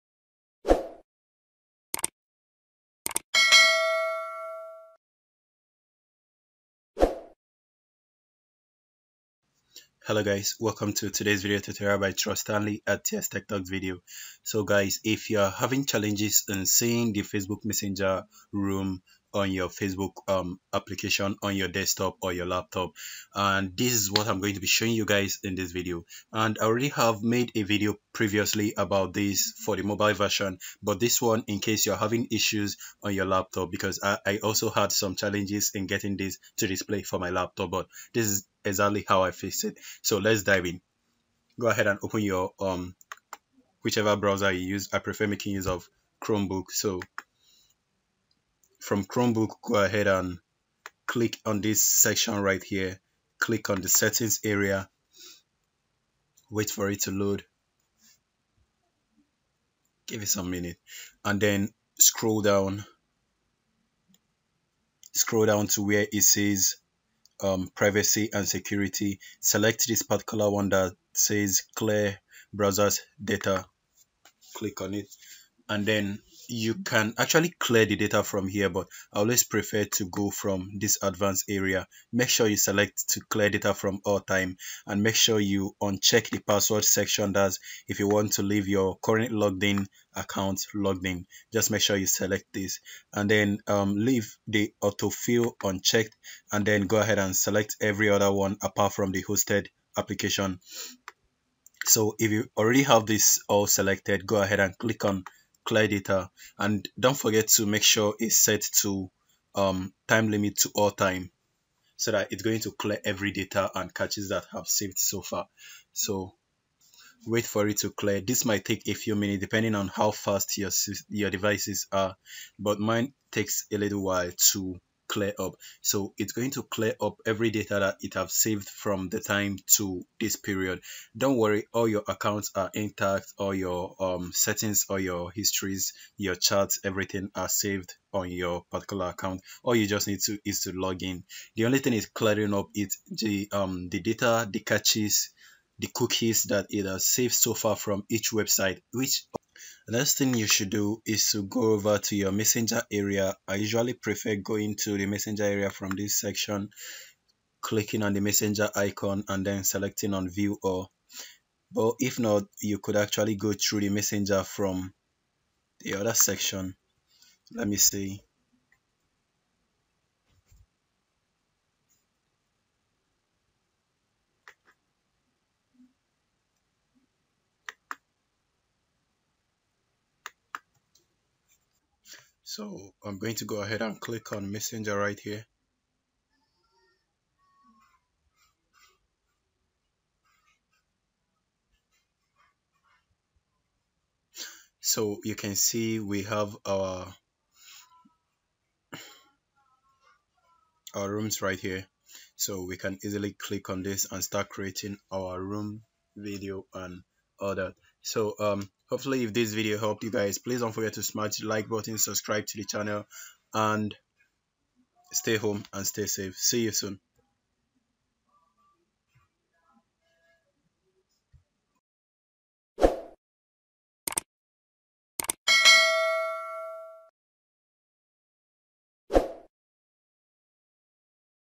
how to get facebook messenger room on desktop
AM going to show you how i got the Facebook messenger room to display on my laptop after some trying i finally have the Facebook messenger room on my laptop, hope this helps you too.